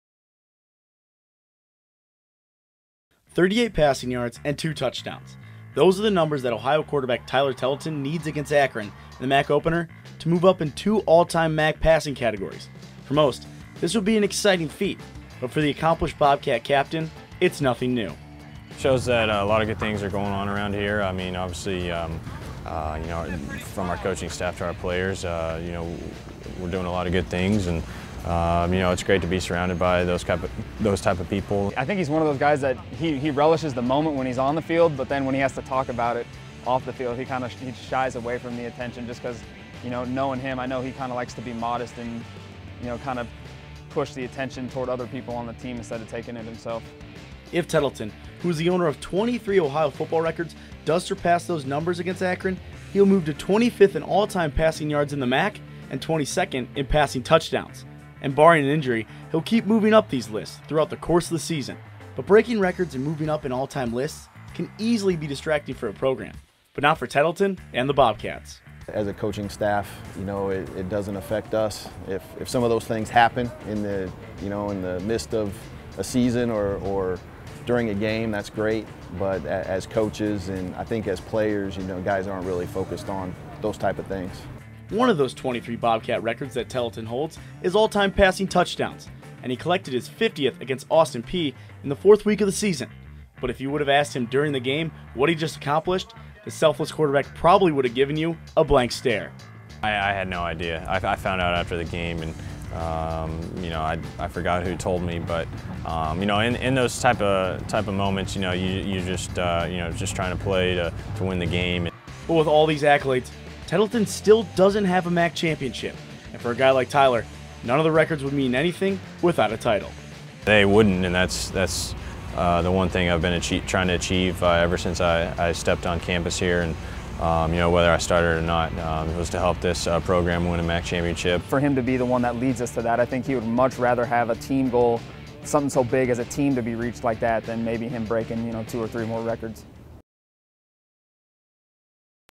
Ohio Football: Records Keep Falling To Tyler Tettleton
Bobcat Sports Showcase reporter Patrick Moore breaks down starting quarterback Tyler Tettleton's career numbers and looks at just how many records might fall before the senior is done this season.